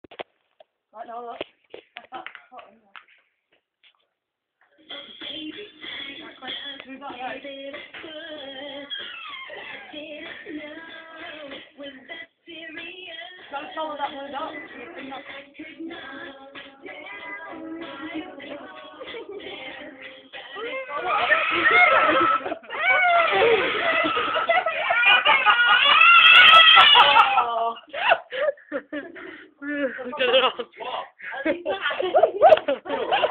funny kid scared
my nephew scared to death by the scary maze thing